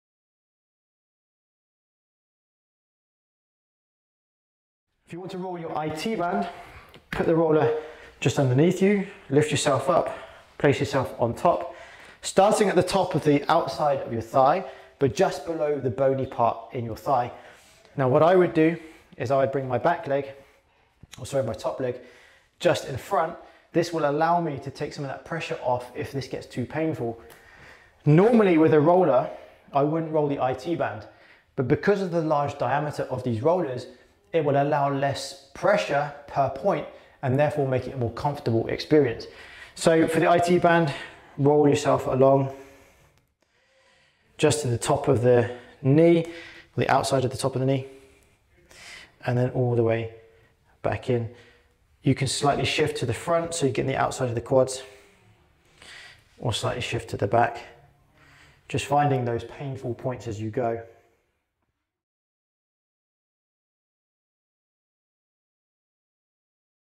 How to Release Your IT Band | Fitbeast Back Roller
In this video, we will be covering ways how to release your IT Band with Fitbeast Back Roller.

Back rolling is one of the most common and effective methods of relieving muscle tension, myofascial release, and deep tissue massage. In general, back rolling is an excellent therapeutic tool for relieving tight muscles and creating more space in the body. Back rolling is a great form of recovery for athletes, yogis, and anyone with a sedentary lifestyle.

We also provide a series of back-rolling exercises to relieve lower back pain, keep up the good work by making back-rolling a part of your wellness routine. You’ll continue alleviating tension and creating space in the body, and as a result, you’ll reap the benefits of reduced pain, increased range of motion, and a greater sense of well-being overall.